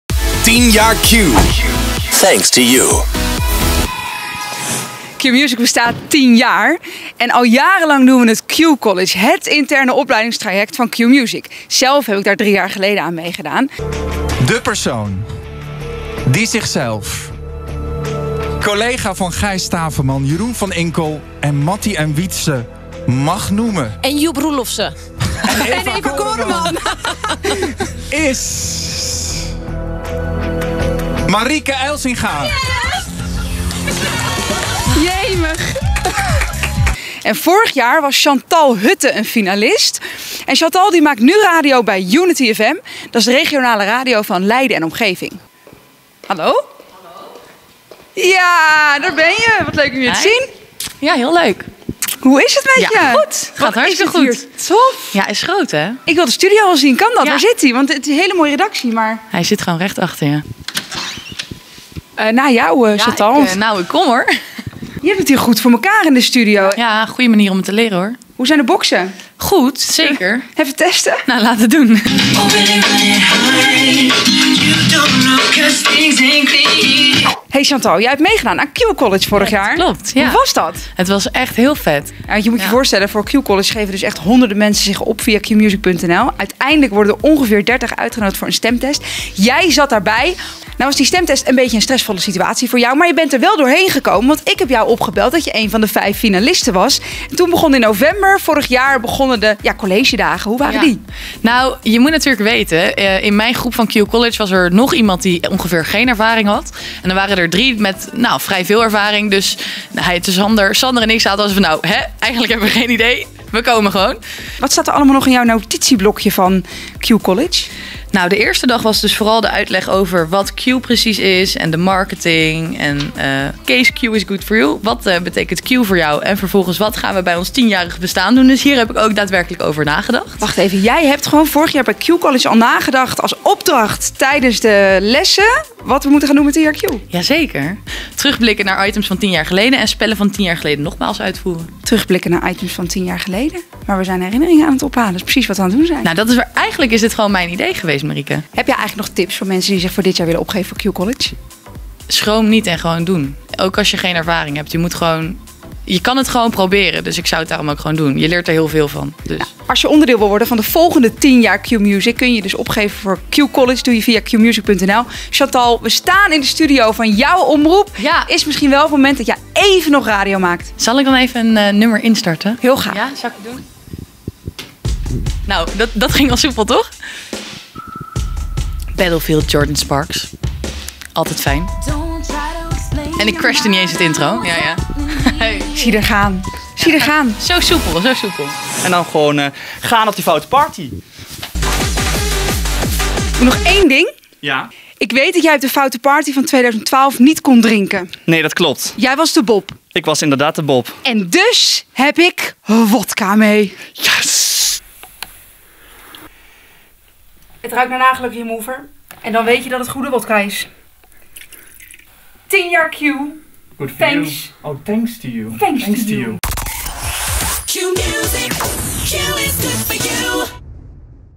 Waar het ooit begon... // Q-report #013 - 10 jaar Q
10 jaar Q - THX to YOU! Q-dj Marieke trekt de komende weken door het land om herinneringen op te halen aan 10 jaar Q-music. Meer? Check

Je hoort Marieke Elsinga ieder weekend van 19 tot 21 uur op Q-music.